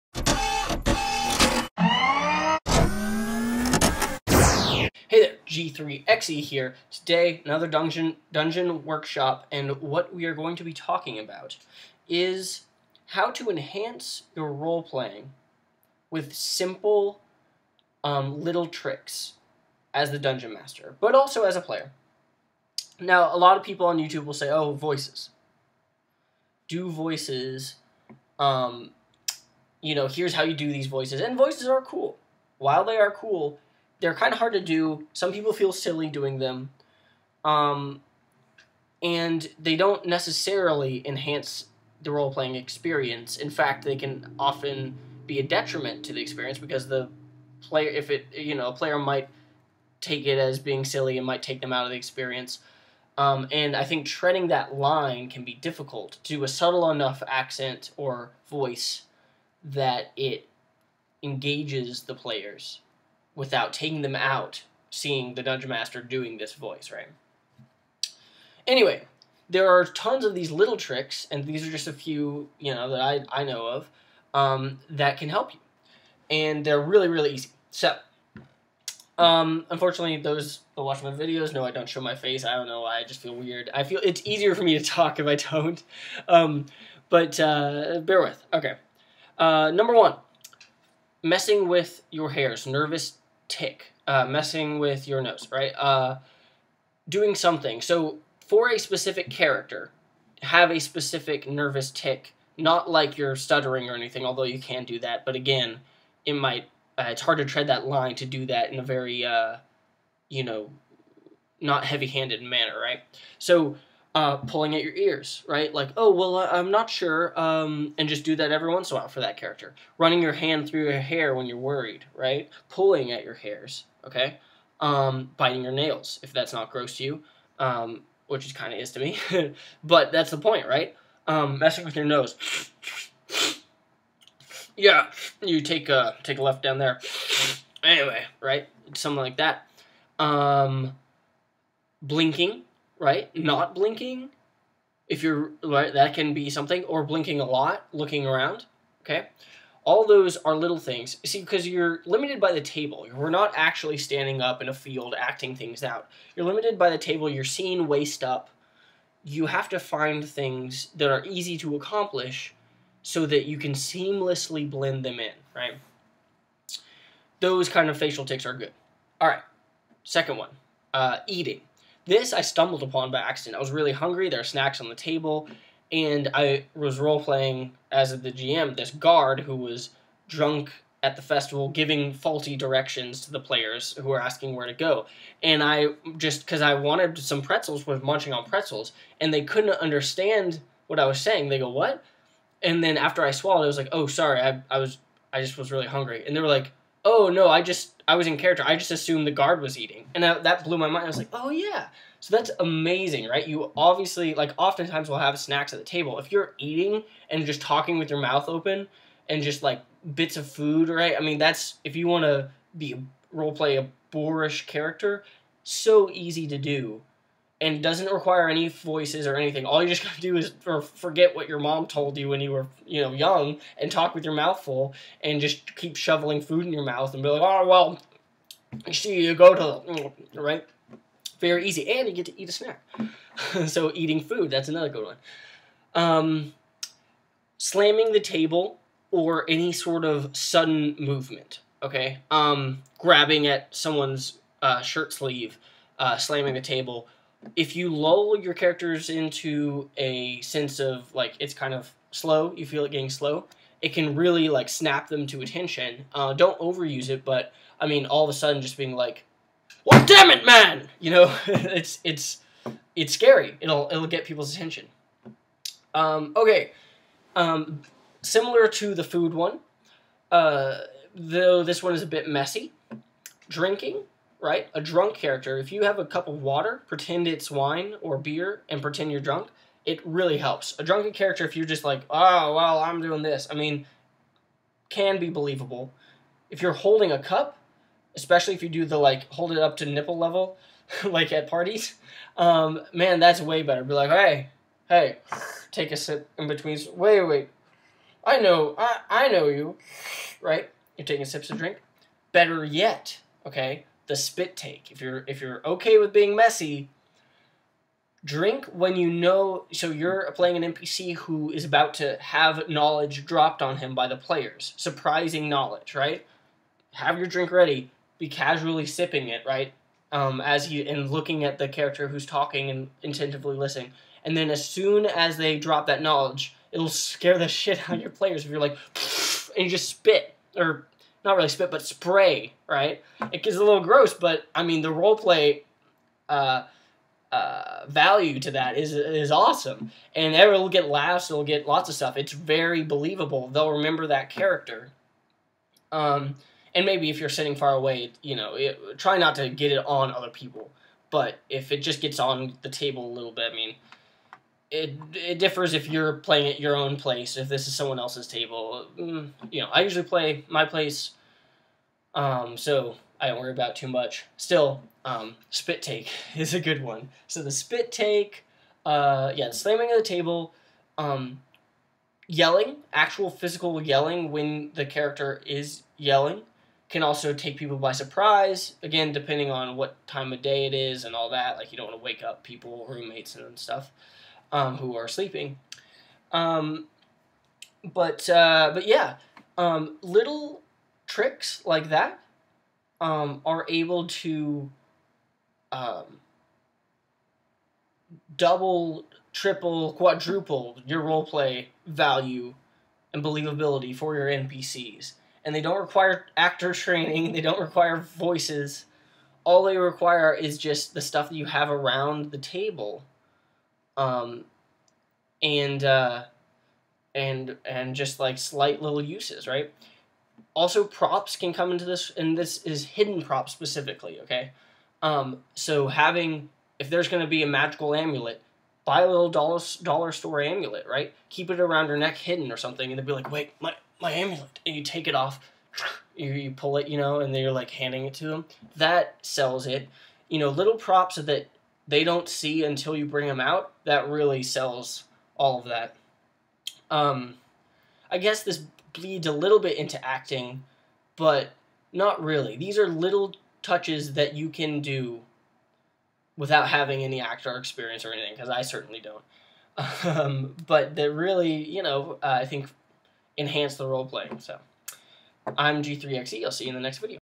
Dungeon Workshop - Easy Ways to Double Your Role Playing Skill
In this edition of the illustrious series Dungeon Workshop, we're taking a look at ways, beyond doing voices, to enhance your role-playing skills!